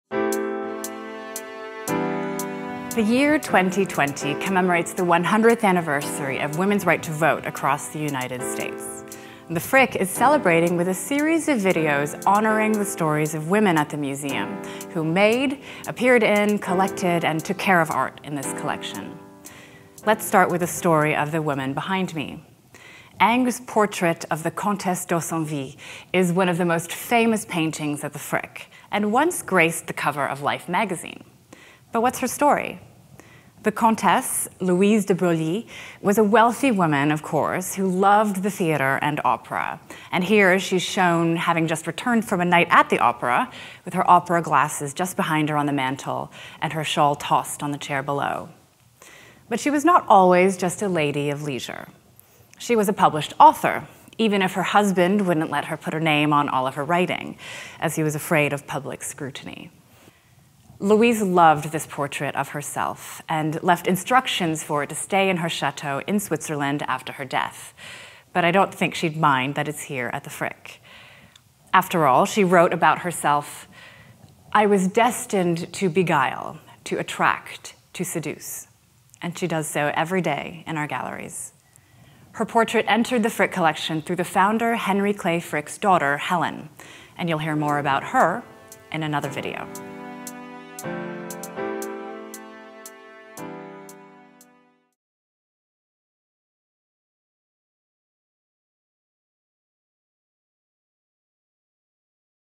Comtesse d'Haussonville | What's Her Story?
The year 2020 commemorates the one-hundredth anniversary of women’s right to vote across the United States. The Frick is celebrating with a series of videos honoring the stories of women at the museum who made, appeared in, collected, and took care of art in this collection.

Director/Editor/Producer: Lisa Candage Goble
Director of Photography/Camera: George Koelle
Sound Production: Sean Troxell
Lighting: Sean Troxell and David Morneau